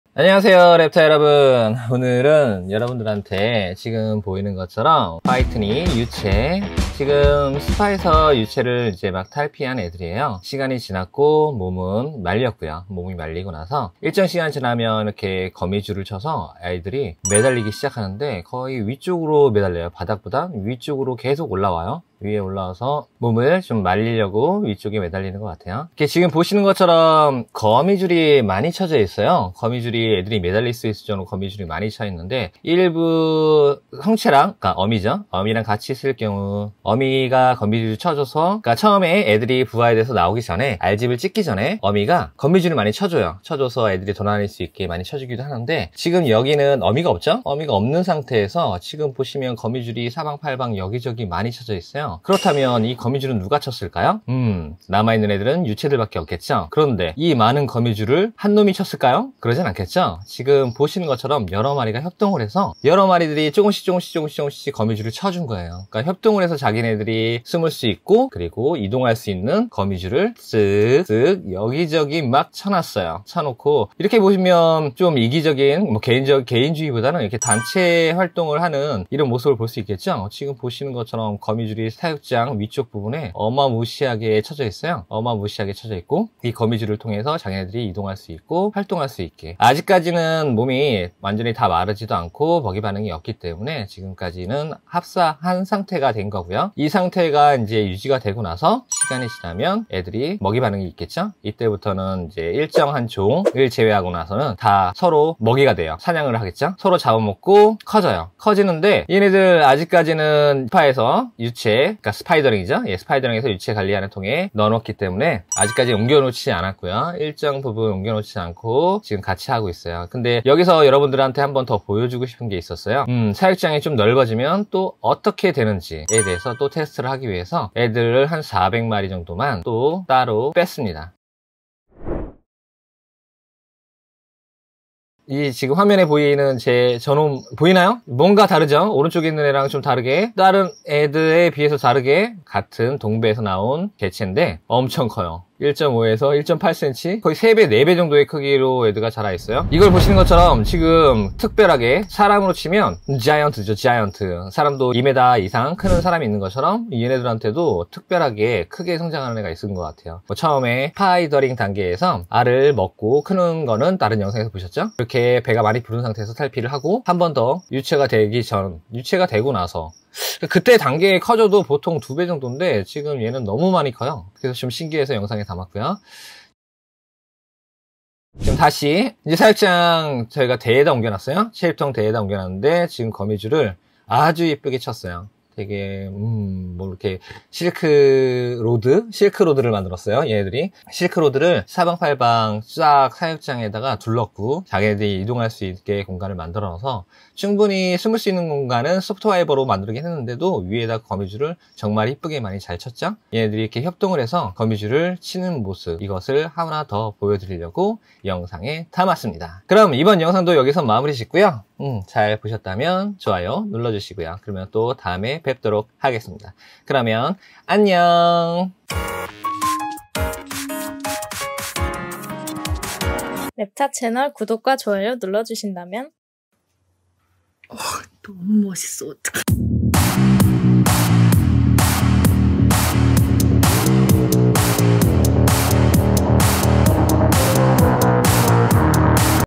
How Do baby tarantulas Make Their Webs? and Giant tarantula a baby?
스파이더링에서 유체까지 단체로 사육장에 넣어 두었을 때 생기는 일.
독특한 유전자를 가진 거대 유체 등장...

Acanthoscurria geniculata
Brazilian Giant White Knee